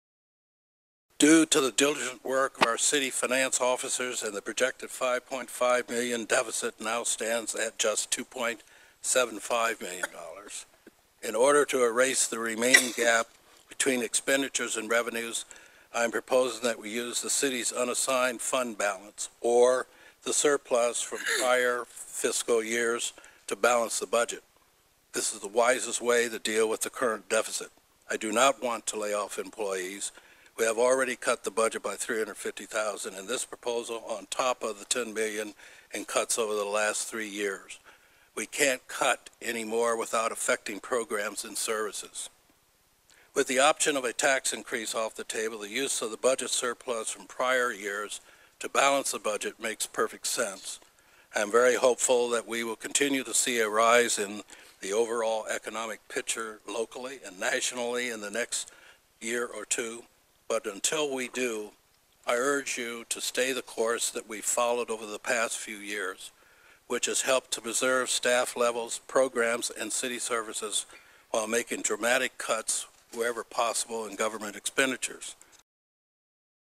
Addressing Wilmington's Budget- part 4
Wilmington Mayor James Baker (D), during his 2013 Fiscal Year Address, offers his solution to closing the city's budget gap.